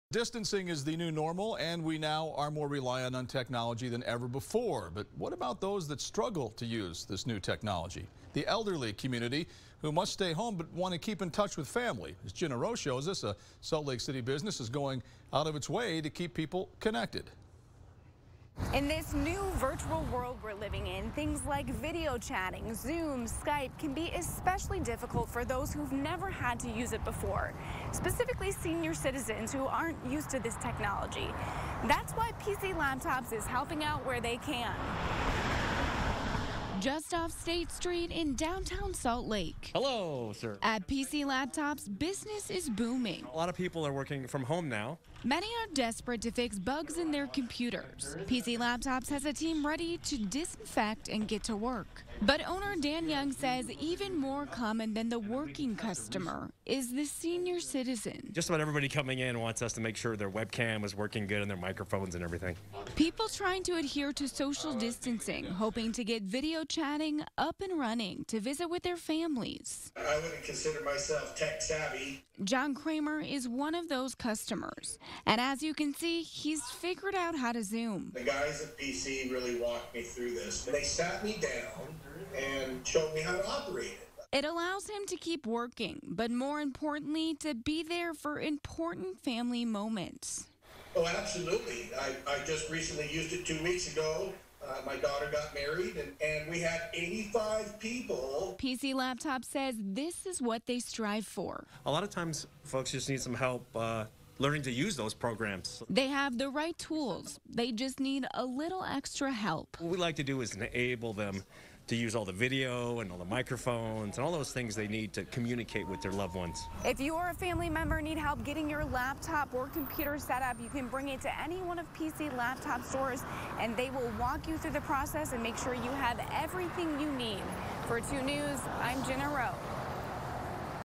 PC Laptops helps seniors get set up in virtual world
With social distancing as the new normal, people are now more reliant on technology than ever before. But what about those that struggle to use it? PC Laptops is helping seniors get their computers up and running.